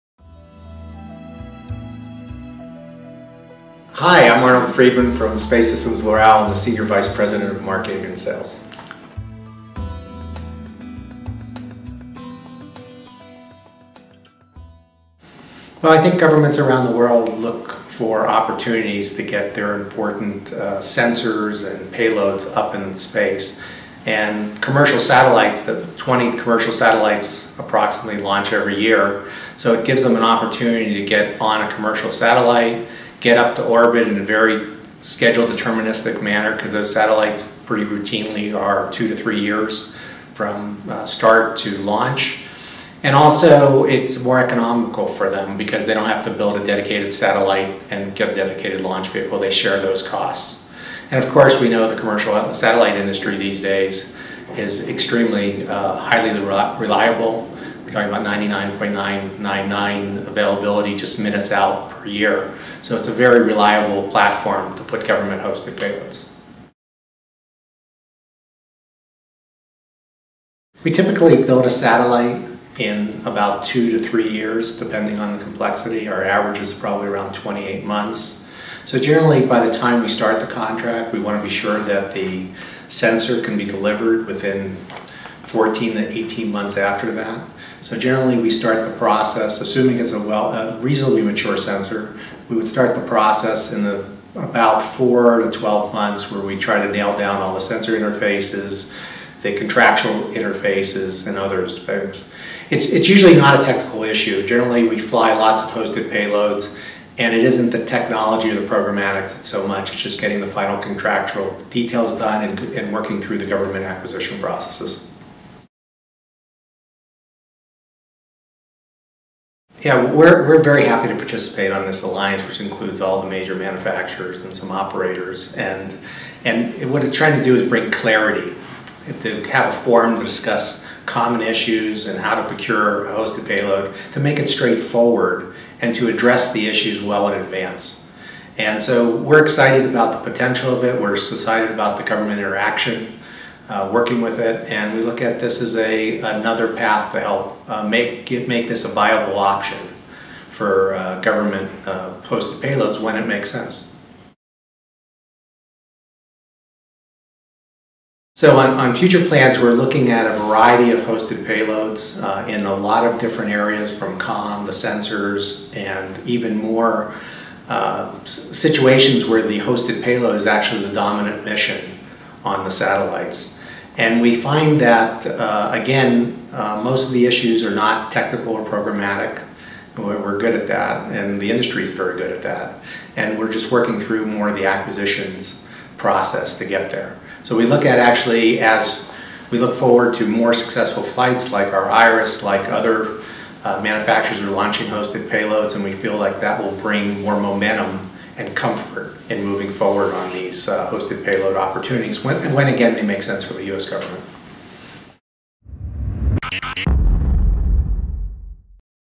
Arnold Friedman, Space Systems/Loral
Space Systems/Loral VP of Marketing & Sales, Arnold Friedman, met up with hostedpayload.com during the National Space Symposium. Friedman discusses why Space Systems/Loral is helping manufacture hosted payloads and why the European Space Agency is getting involved. Friedman also explains the Hosted Payload Alliance and the results he'd like to see both for Space Systems/Loral and the Alliance as a whole.